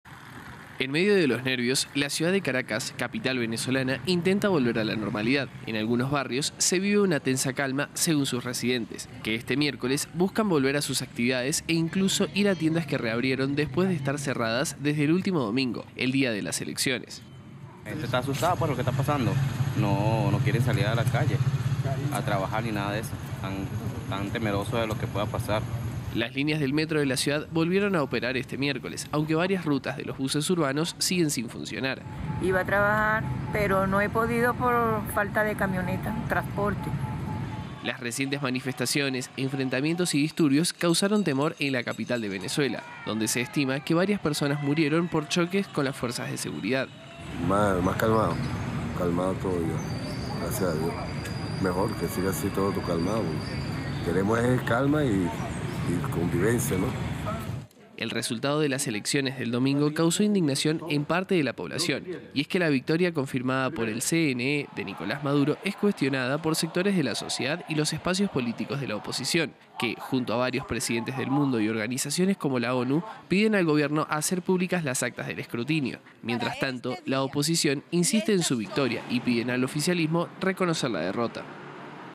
Caracas intenta volver a la normalidad tras días de protestas por el resultado electoral
En medio de la tensión desatada tras los comicios presidenciales en Venezuela, muchos ciudadanos de Caracas intentan volver a la normalidad, en medio de una calma que podría romperse en cualquier momento, luego de las protestas que dejaron varios muertos. Los negocios y centros comerciales poco a poco vuelven a abrir sus puertas, aunque el sistema de transporte público todavía presenta limitaciones.